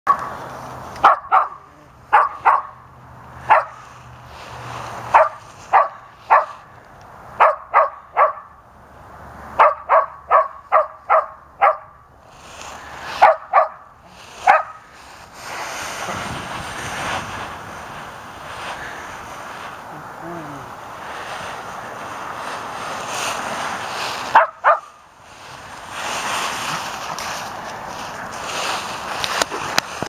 I found you
Sugar and Charlie playing hide and seek in the leaves